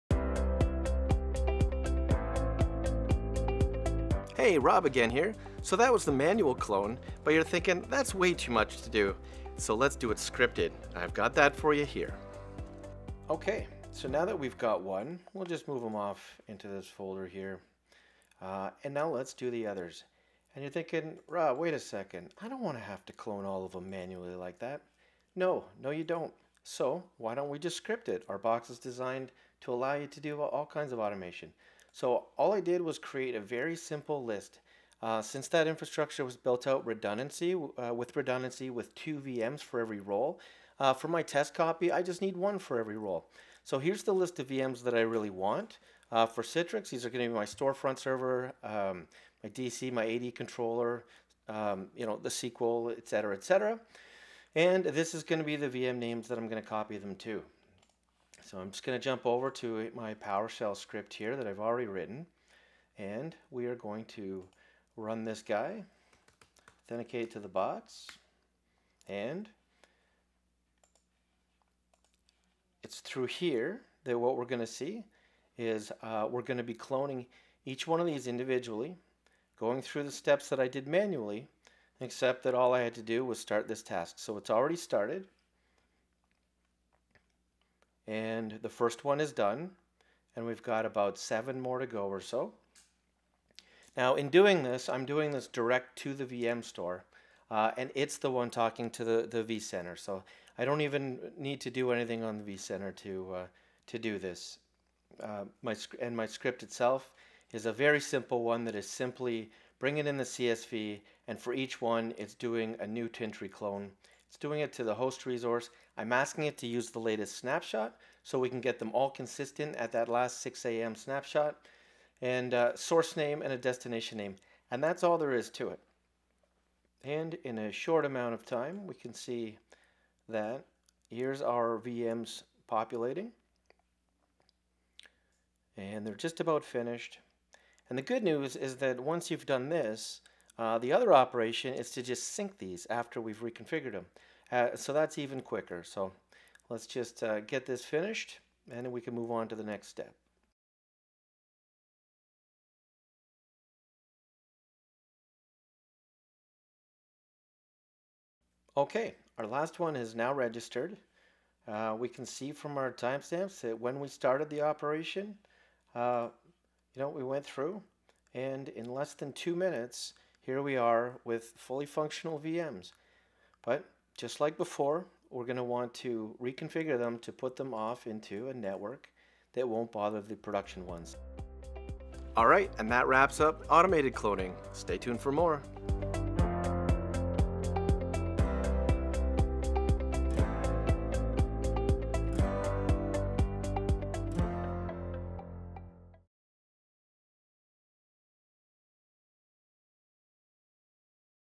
Automating cloning VMs with scripts (2 of 6)
This series of demos of Tintri SyncVM shows the simplicity of cloning VMs, isolating them in a test environment and refreshing them from production—when you have VM-aware storage. In video 2 (of 6), we demonstrate how to use scripting to automatically clone a group of VMs in less than 2 minutes.